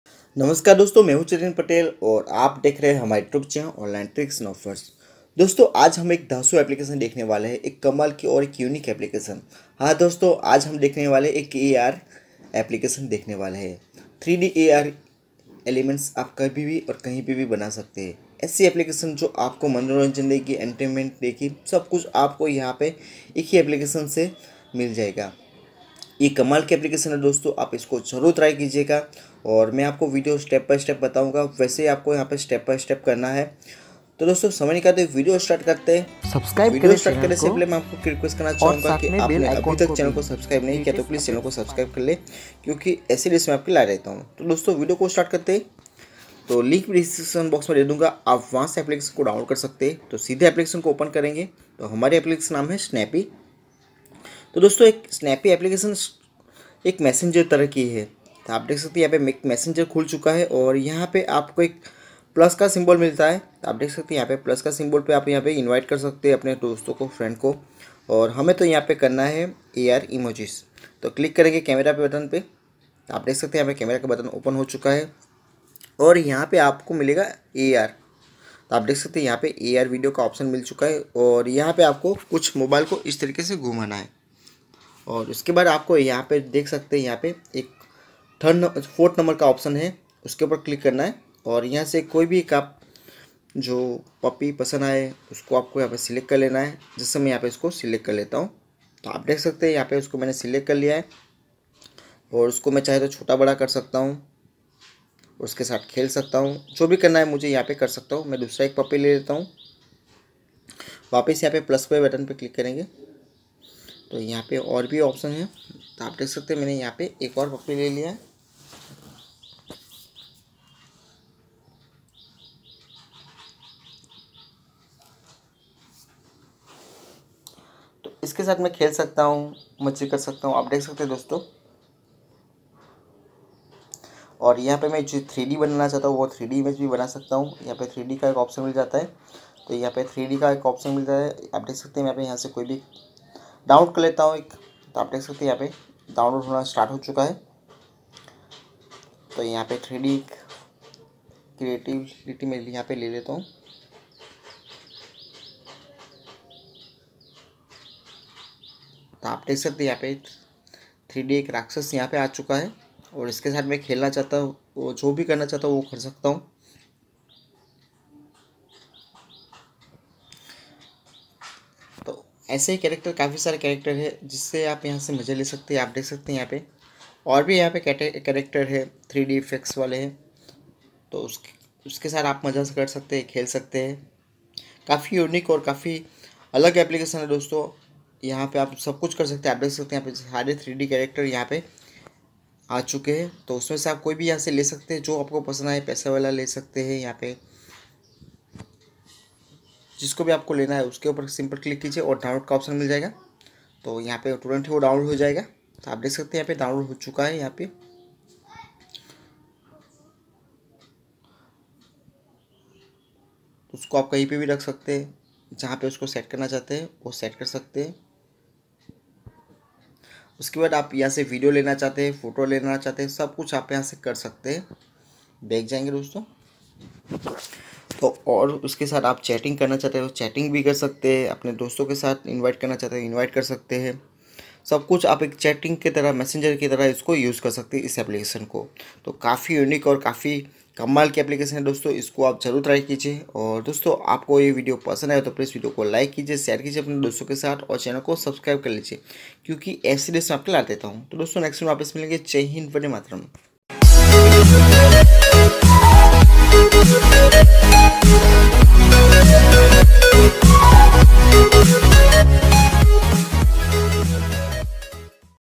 1 MOST USEFUL Augmented Reality (AR) Apps 2019 !! By Online Tricks & Offers.
1 MOST USEFUL Augmented Reality (AR) Apps 2019 !! By Online Tricks & Offers.

Features :
• AR VIDEO EDITOR: Create cool, short videos using Augmented Reality technology. Use huge libraries of 3D objects, 2D images, gifs, stickers and other types of content you can select from. Control everything in real time while recording the video to get unique results.

• PERSONALIZED VIDEO FEED: Scroll through tons of unique and cool videos the Snaappy community have created which were pre-selected just for you.

• GEO AR MESSAGES: Do you also think that location is important? Now you can create an AR message and hide it at a specific place. Your friends will have to visit the original location in order to see it.

• AUGMENTED REALITY MESSAGES: Create special 3D AR messages using Snaappy characters, images, videos and text. Your friends would be able to see it in FULL 3D at their own location.

• LIVE VIDEO EDITOR: Record a video and use our unique video effects to enhance your experience in real time.

• IMAGE EDITOR: Did you know that your photos can talk? Take a photo and add a 3D character, special sound effects and your own voice to generate a cool short video clip you can share with the world.

• REAL TIME TYPING: Hate waiting to see what (...) means when friends are typing a message? Skip the wait with Snaappy and see what your friends are typing, as they type, in REAL TIME!

• 3D CHARACTER MESSAGING: Go beyond regular emojis when expressing yourself with Snaappy animated characters, add a voice over when sending a character or let Snaappy’s algorithm predict the right character based on your personality.

• AR PERSONAL PROFILE: Store your videos, AR content and images in one place. Other Snaappy users can comment and “like” your content.

• SECURE MESSAGING: Use Snaappy disappearing messages keep your messages, pictures, videos and chat histories 100% safe and private.

Online Tricks & Offers
Technology Update, Tech News, Unboxing, Review, Mobile Solutions & etc.